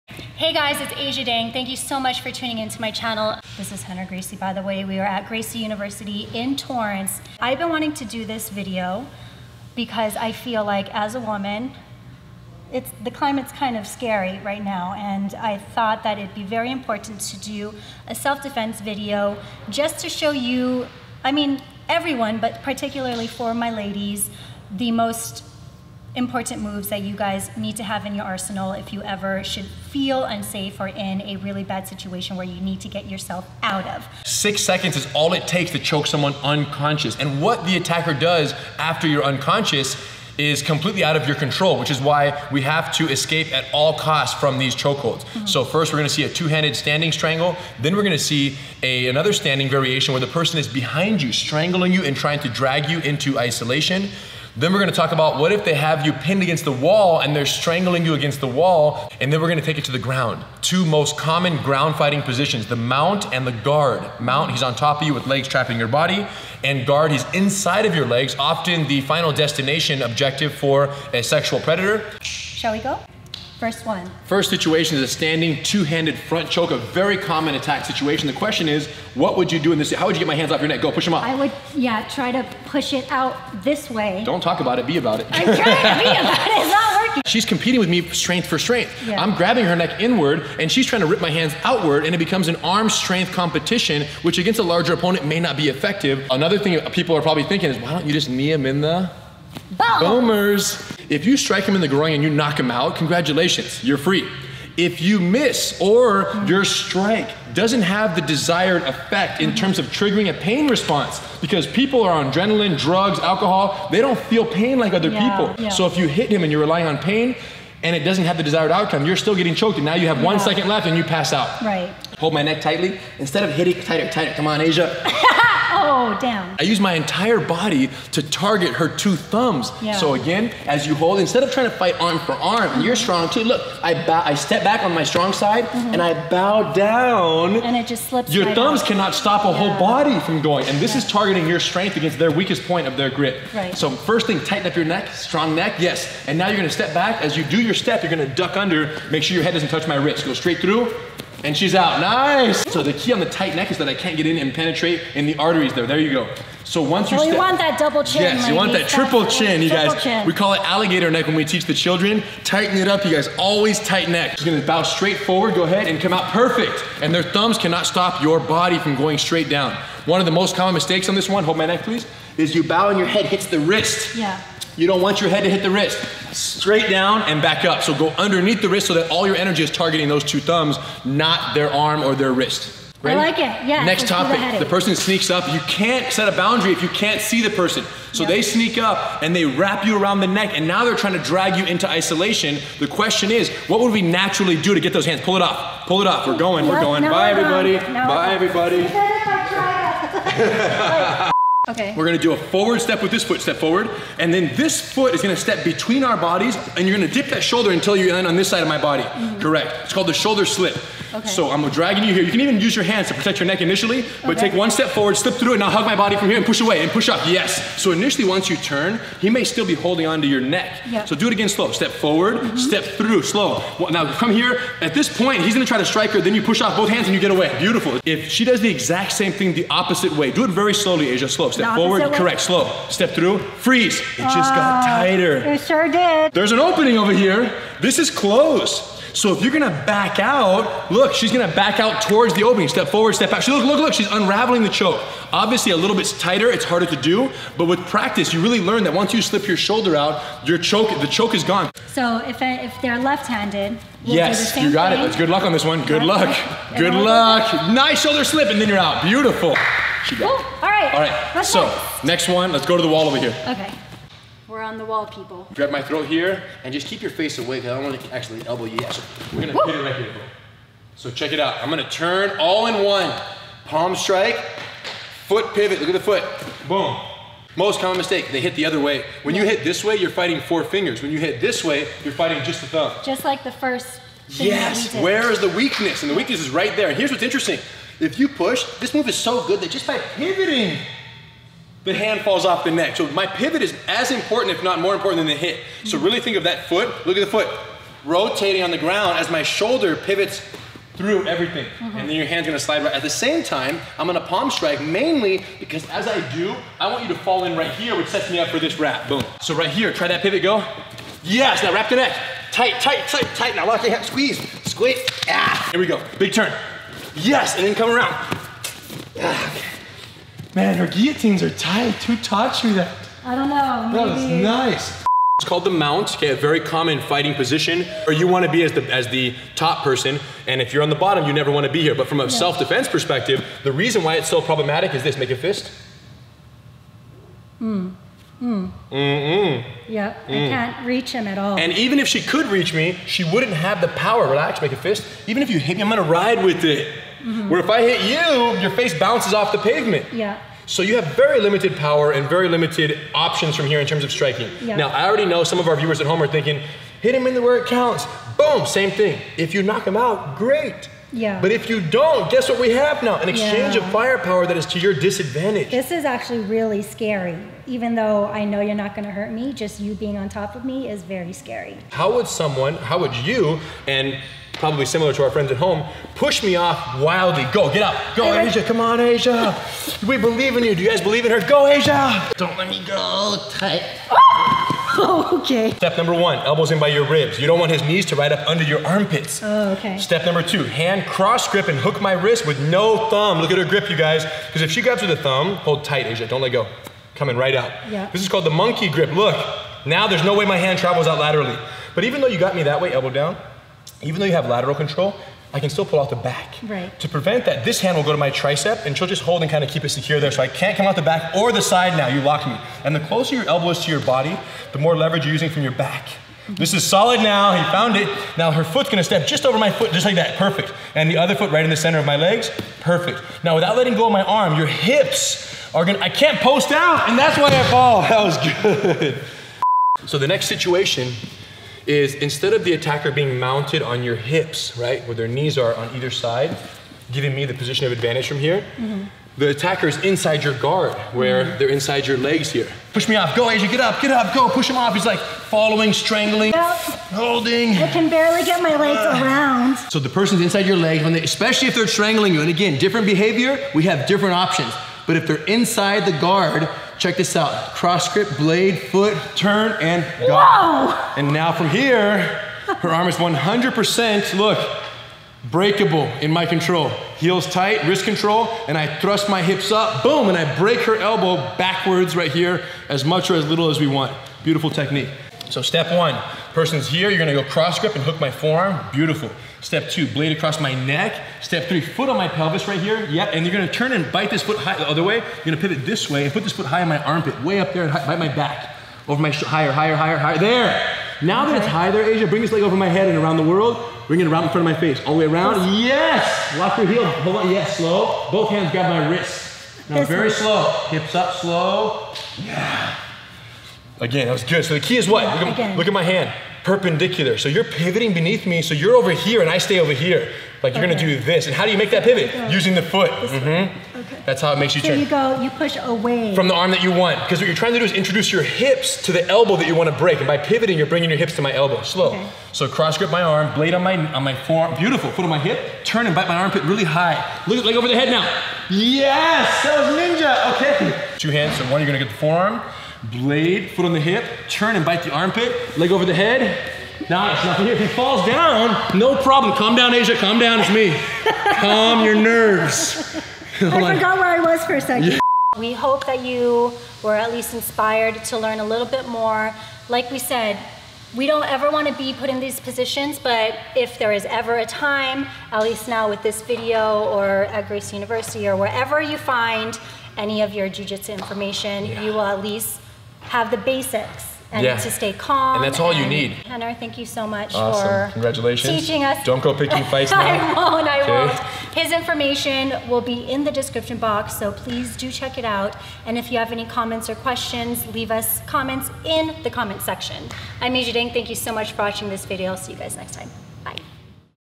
5 Choke Hold Defenses Women MUST Know | Self Defense | Aja Dang
I'm Aja Dang and I teamed up with Renner Gracie from Gracie University to show you 5 choke hold defense techniques every woman MUST know when involved in a fight. In a perfect world, women wouldn't need to know self protection, but we are not in a perfect world. This video will teach you how to escape from some attacks and sexual assault positions. We covered some of the best defense moves in this self defense training video - even how to escape a choke hold.

While this doesn't take place of actual jiu jitsu training, these are self defense techniques that can be practiced at home. I really hope that we never have to use these techniques against an attacker, but better to know the basics than know nothing at all.

* special thanks to Renner Gracie for spending the afternoon teaching us these important techniques.

•Gracie University•